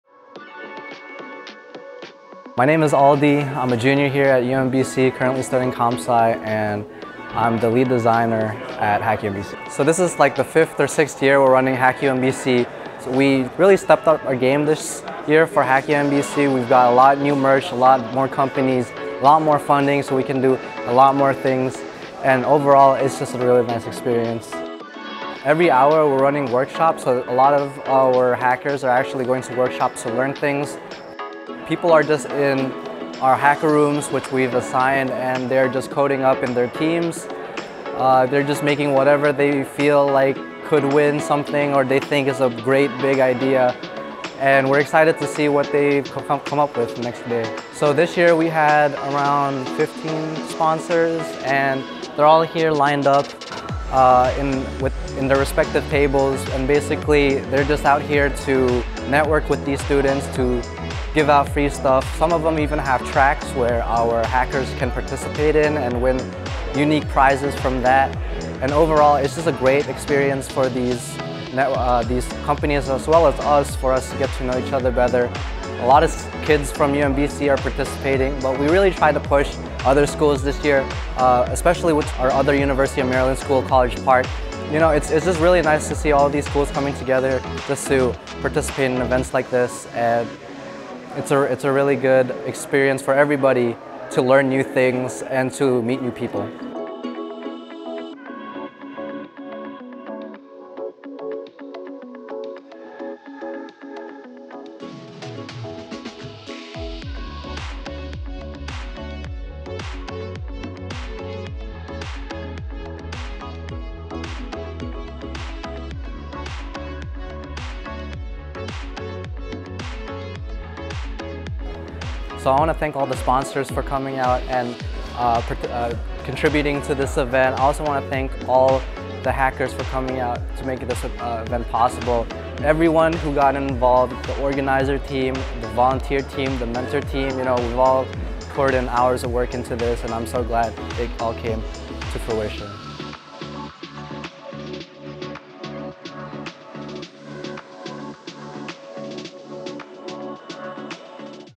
HackUMBC
This year's hackathon was HUGE, welcoming over 1,000 participants from up and down the East Coast. Congratulations to HackUMBC on another tremendous event.